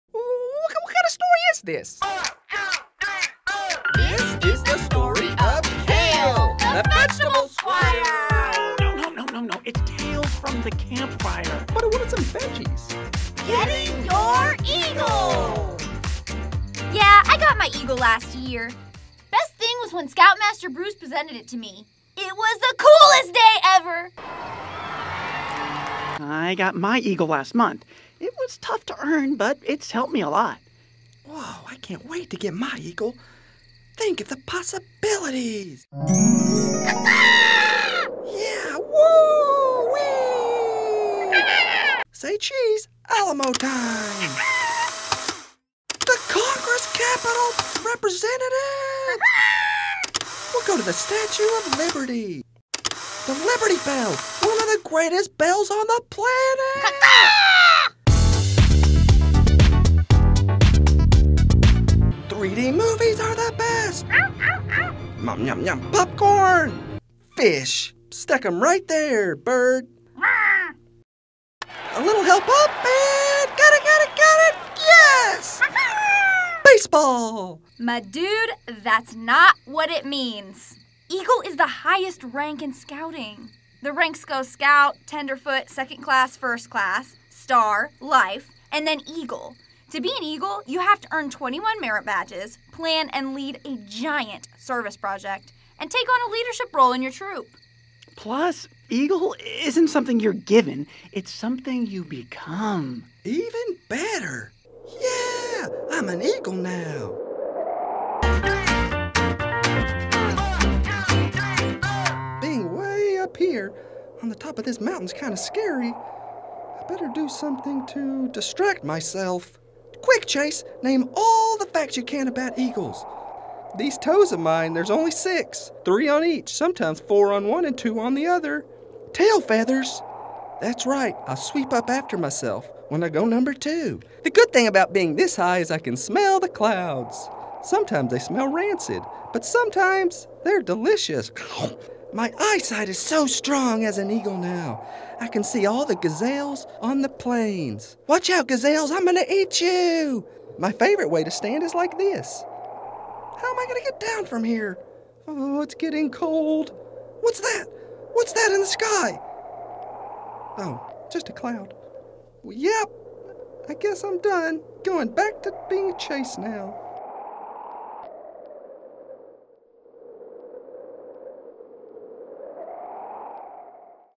Tales from the Campfire: Get Your Eagle! Be an Eagle!
The possibilities are limitless when you get your Eagle. Your Eagle can help you go places and do things that most people who don't get their Eagle may never go and do.

After you get your Eagle, it will always stick with you, no matter what you do in life. Getting your Eagle is a huge honor.

Nobody looks at you the same after you get your Eagle. Your Eagle will consistently help you stand out in a crowd. Eagles always command respect.

Eagles can be found in Washington, D.C., on the baseball diamond and even in entertainment. There's no limit to how high an Eagle can fly. Some Eagles even go into space!

So never give up. It won't be easy, but keep working toward getting your Eagle. If you persevere, one day you'll be standing before your troop in a special ceremony receiving your Eagle.

And then you can give motivational speeches too.

CAST LIST
Beith: himself
Gavin: himself
Chase: himself
Julian: himself
Jacob: himself

GUEST STARS
Eagle 1: Glen Henley
Eagle 2: Chase

Filmed on location in Vancouver, British Columbia.

No fish were harmed in the filming of this episode.